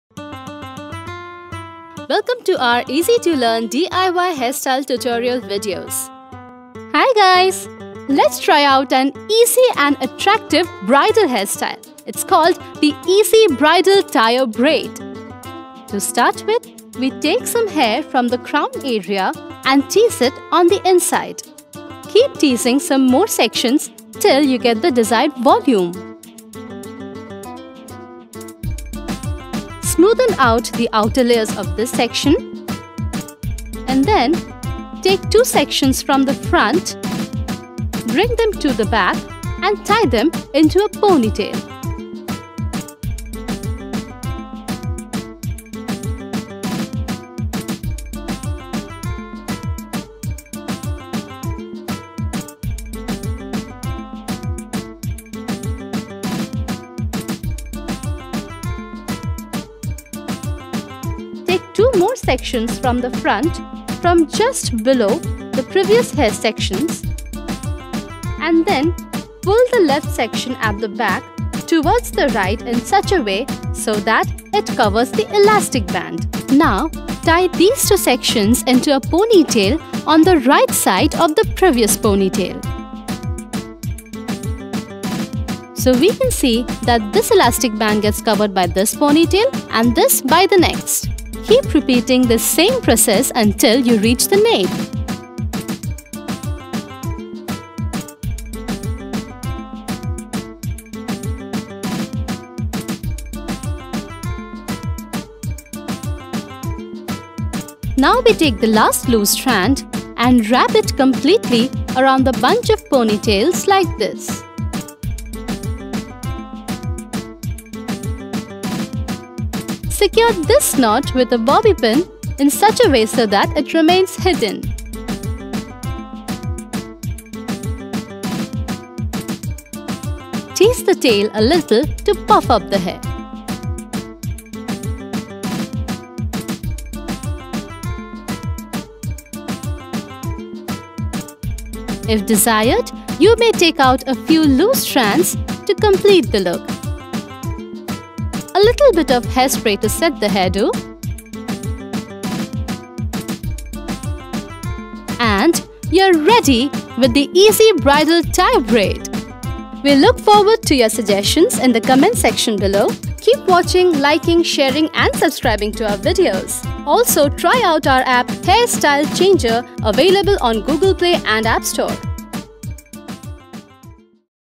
EASY BRIDAL TIER BRAID
This easy bridal tier braid is the perfect combination of classy and elegance.With this easy few steps you get to feel and look like a bride in just three minutes.